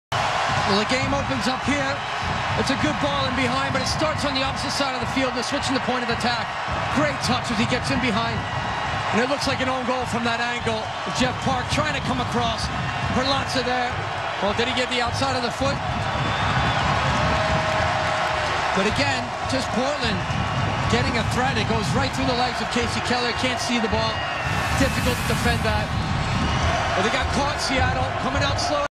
Did Perlaza Score?
Did Perlaza score against the Seattle Sounders, or was it an own goal?